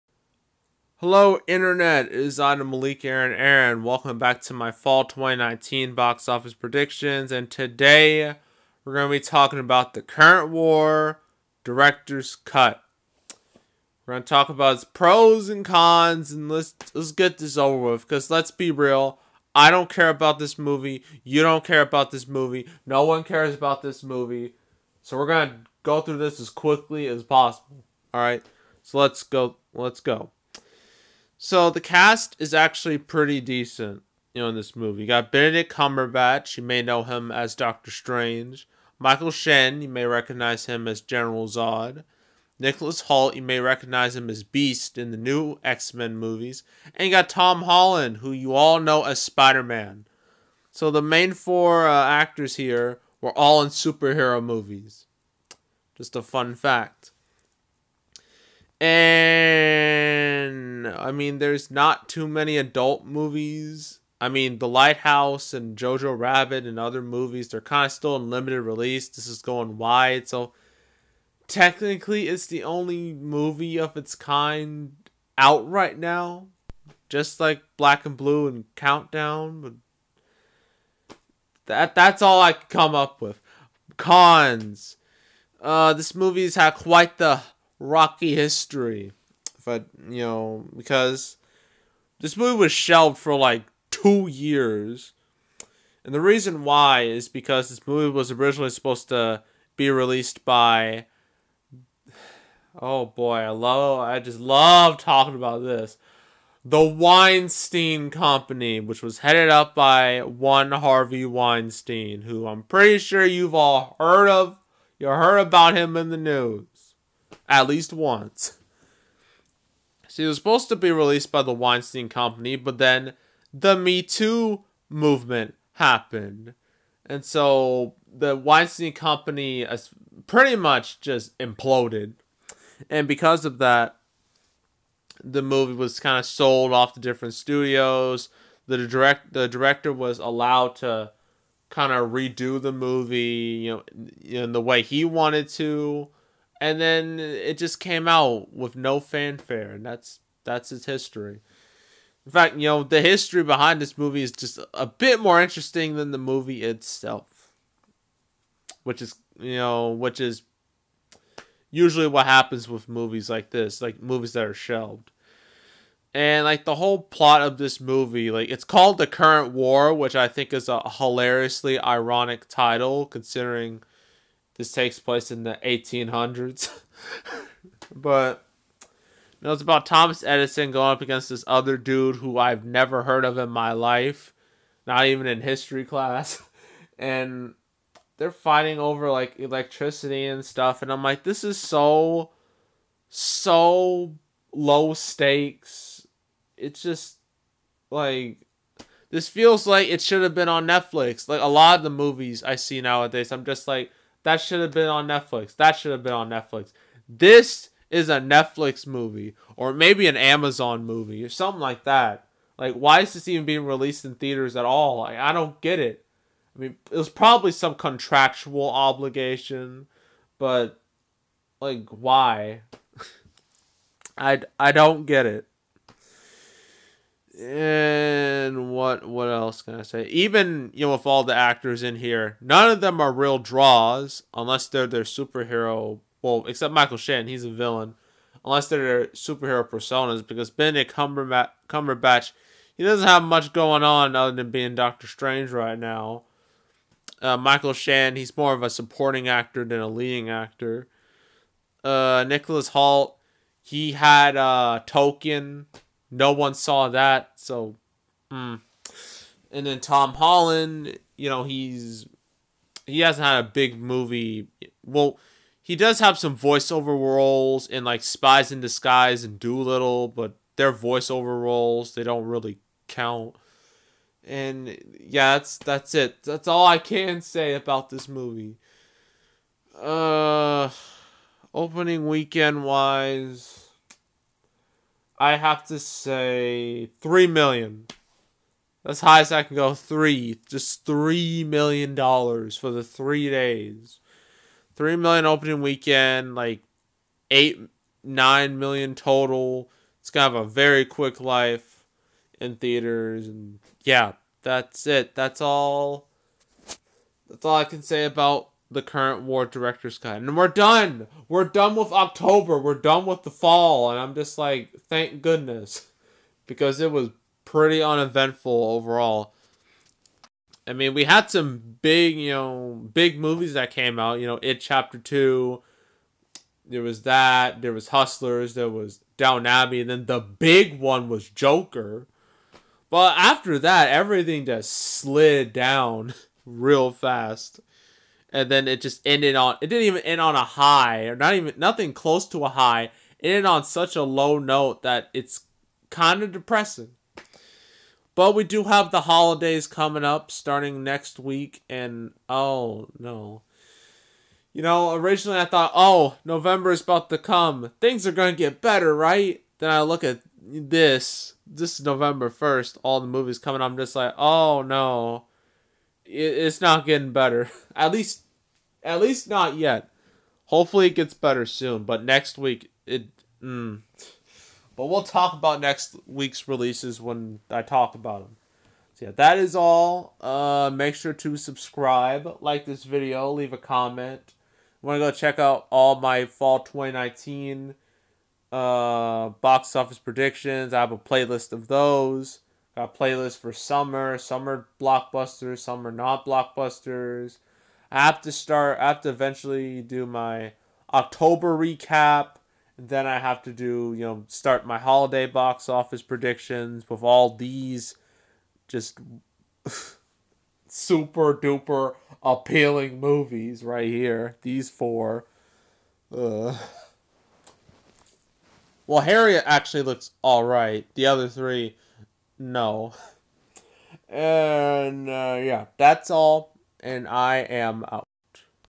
The Current War: Director's Cut Box Office Prediction
Here's my box office prediction for The Current War: Director's Cut.